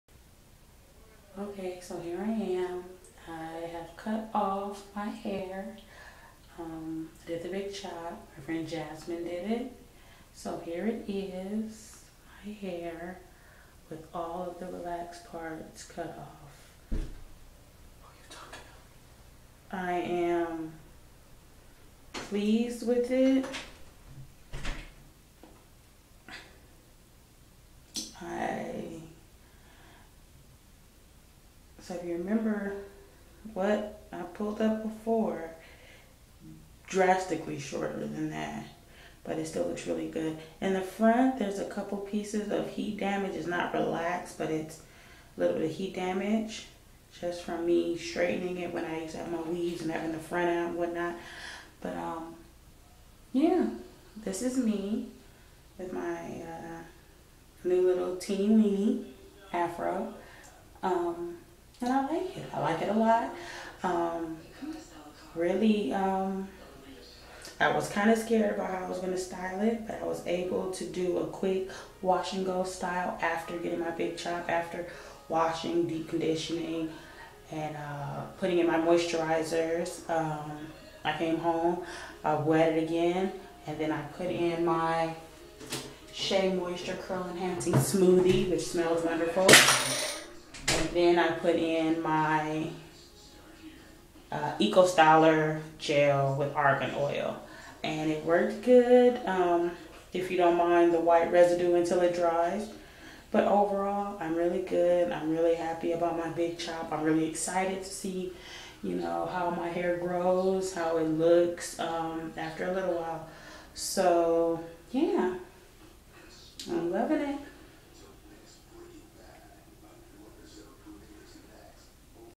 Natural Hair: My Big Chop! 7/22/2012
Here's my video of me an hour after my big chop!
I transitioned (badly) from 3-2011 to 7-2012. Like I said in the previous video: I knew nothing about "transitioning" then.
I still had a patch of heat damaged hair afterwards but I decided to keep it and trim it off little by little.
Sorry for the bad audio, it was an impromptu video for myself (and before I decided to use YouTube) lol.